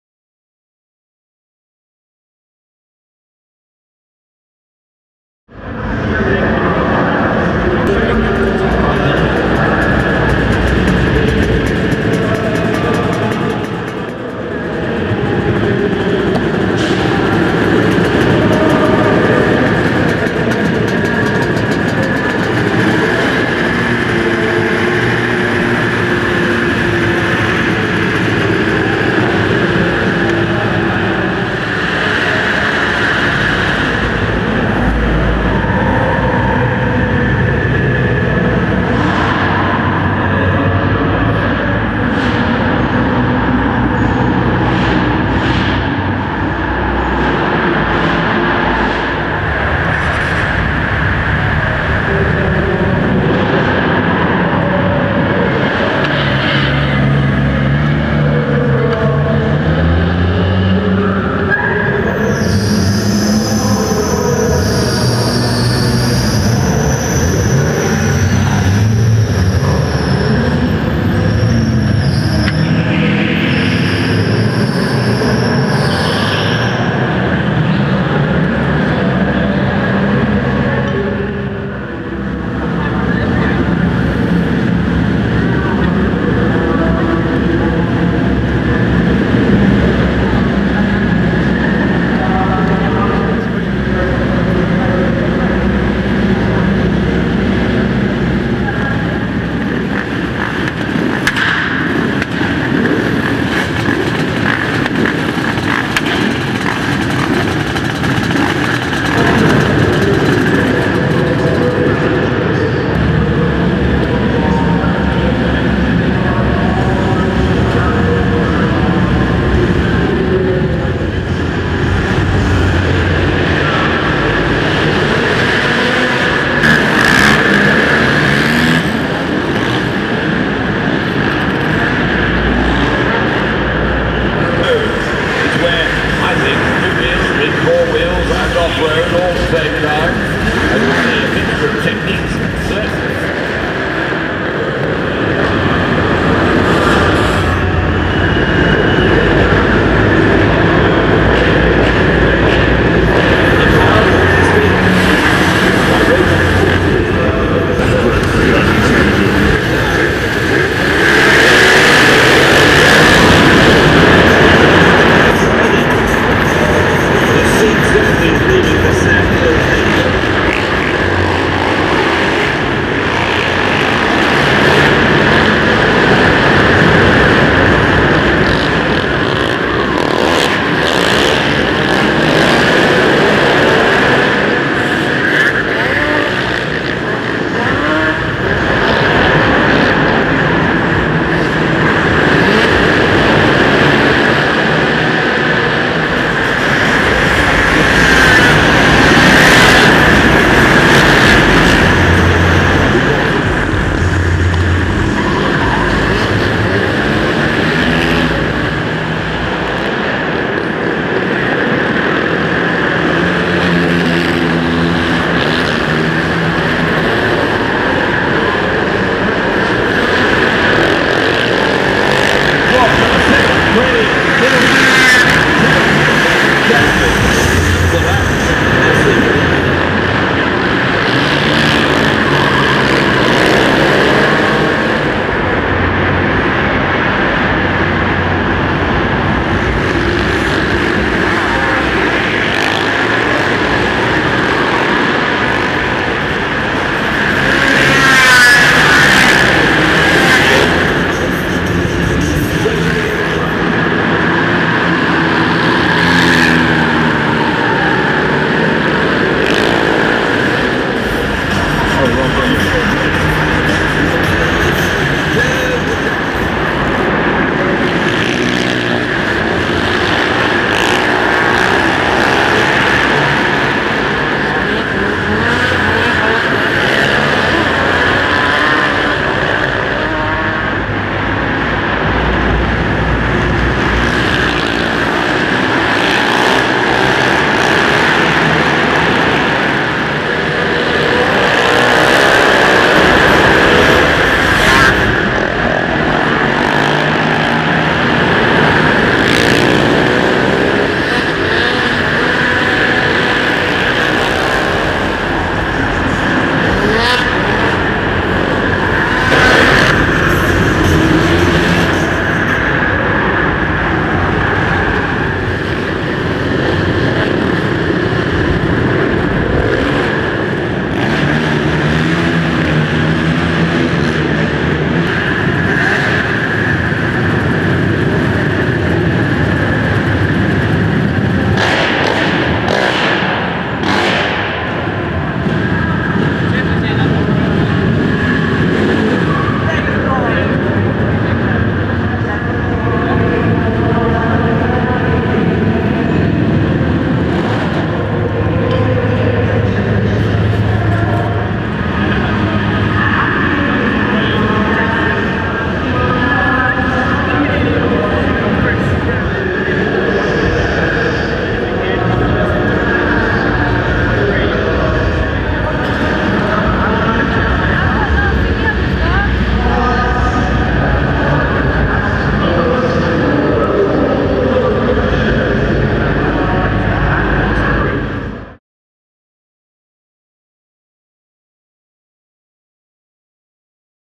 THE SUPERMOTO QUADS HIT THE NEC ARENA (NEW EDIT)
A new tighter edit of the recent visit by the UK Supermoto Quads to the NEC Live Action Arena as part of the Autosport International 2013 show.

These guys will be part of the UK Supermoto series beginning in March.